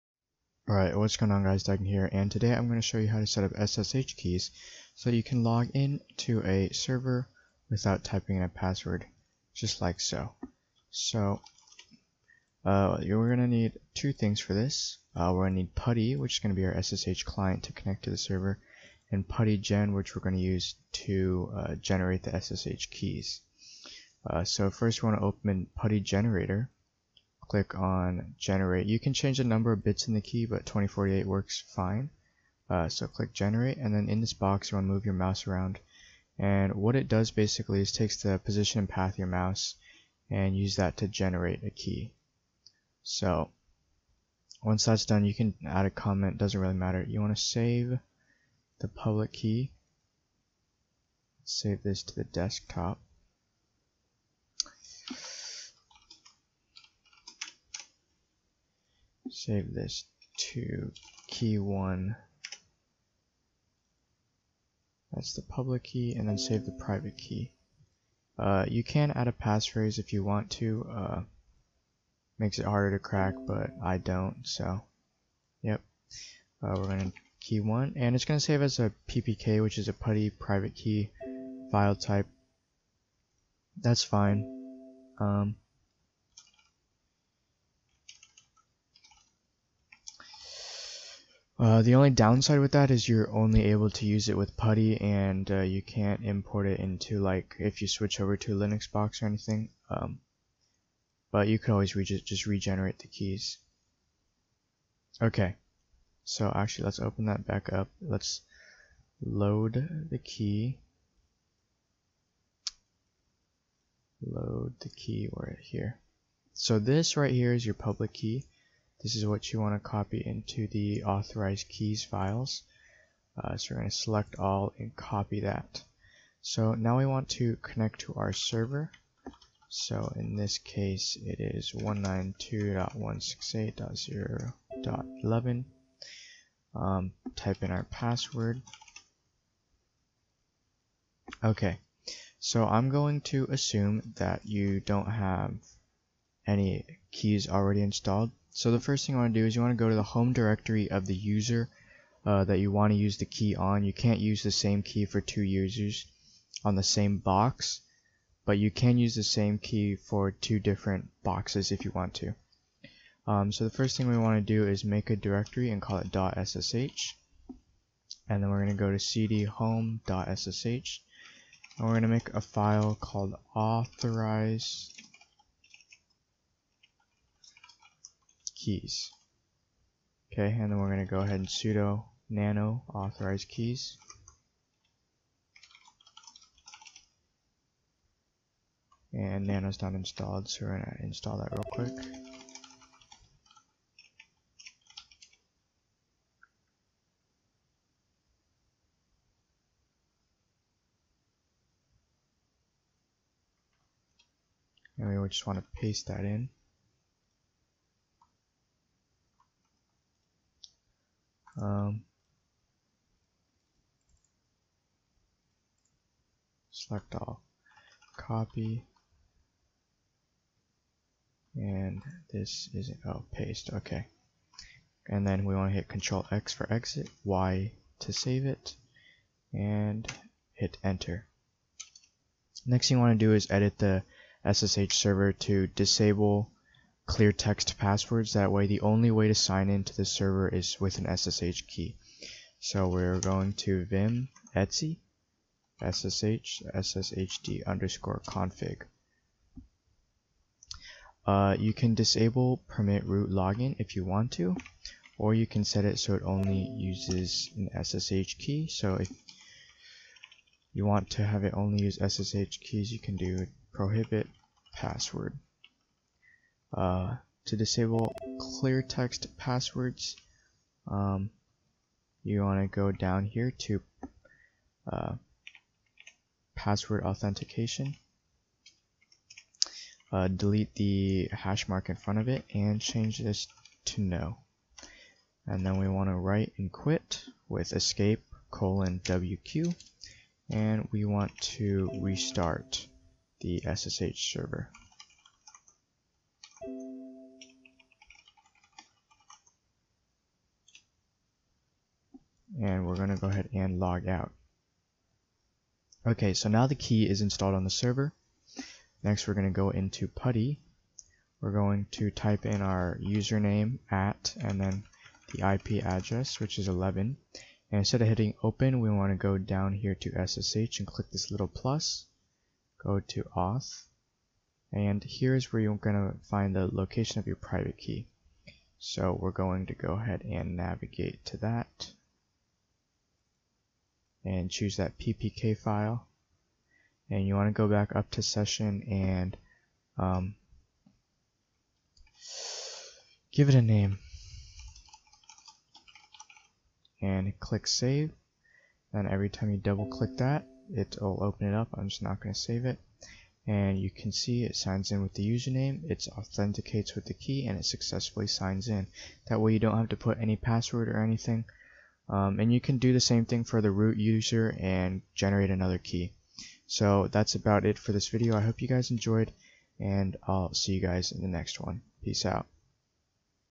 Setting up SSH Keys on Windows using PuttyGen
In this video, I show you how to setup SSH Keys in Windows with PuttyGen. This allows you to connect to your linux server without having to authenticate with a password. I hope you guys Enjoy!